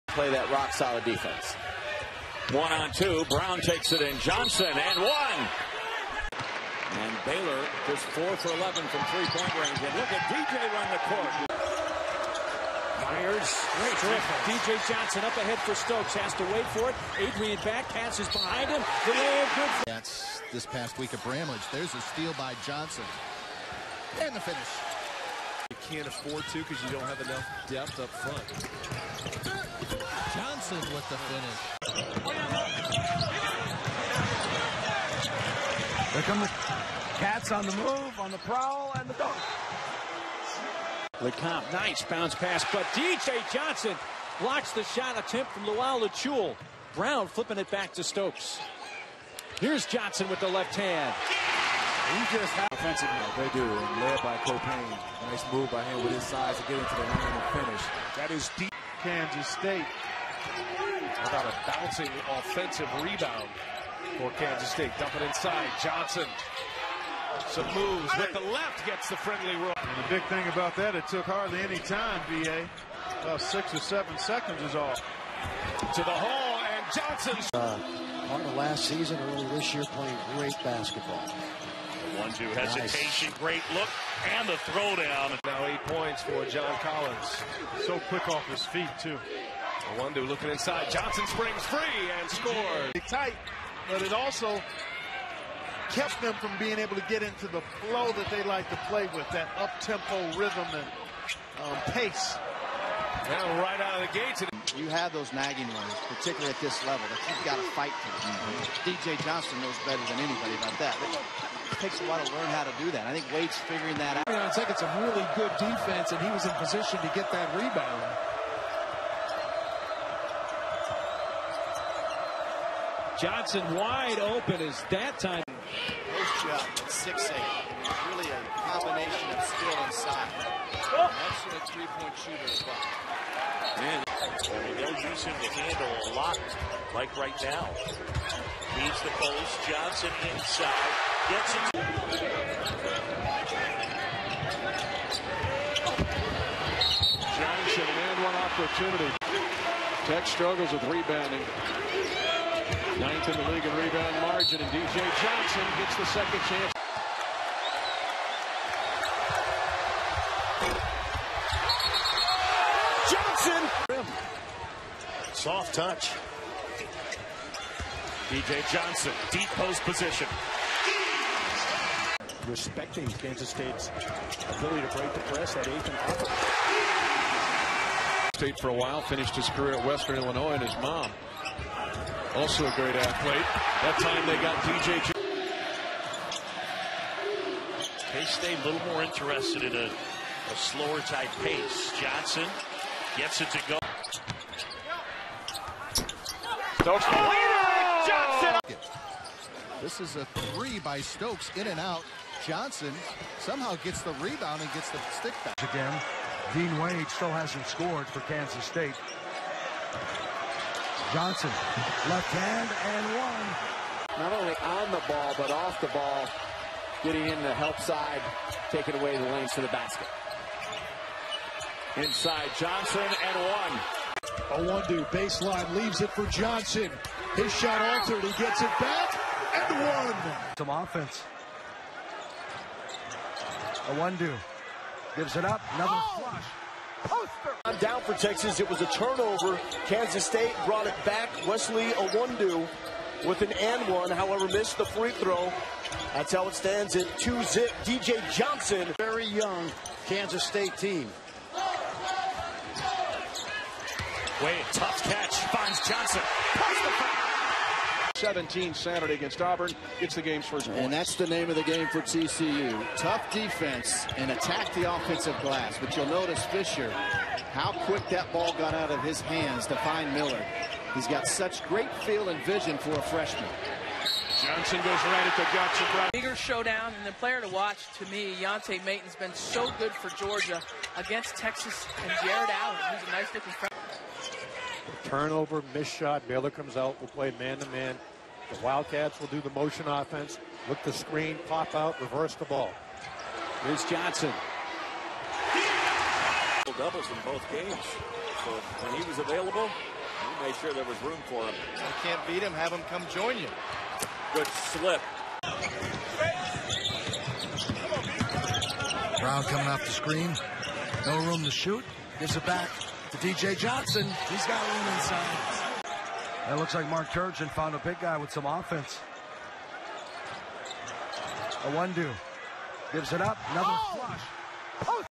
DJ Johnson Senior Highlights Kansas State Basketball 2016-17
KStateMBB's D.J. Johnson's senior season at K-State.